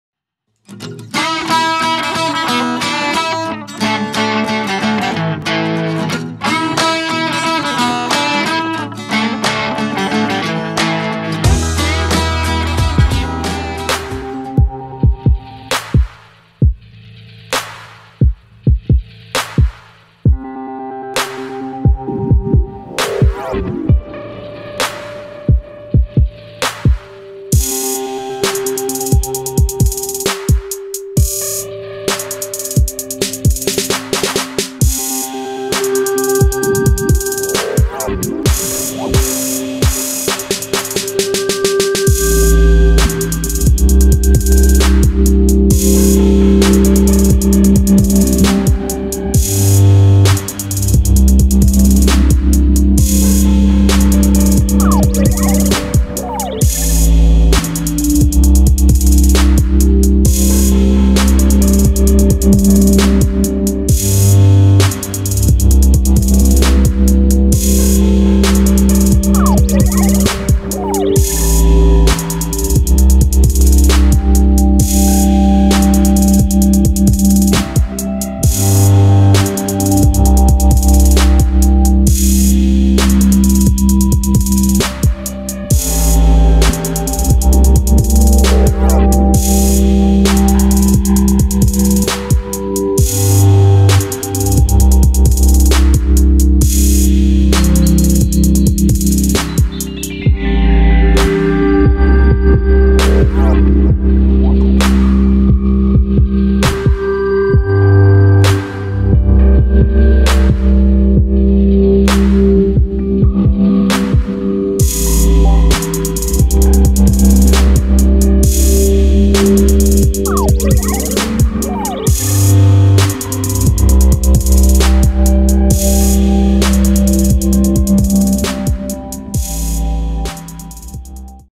Aerial view of Panchapalli Dam Tamilnadu, India. DJI Phantom 4 Drone
My first video of HD footage from Phantom 4, processed in Final Cut Pro X. Panchapalli dam, Tamilnadu, South India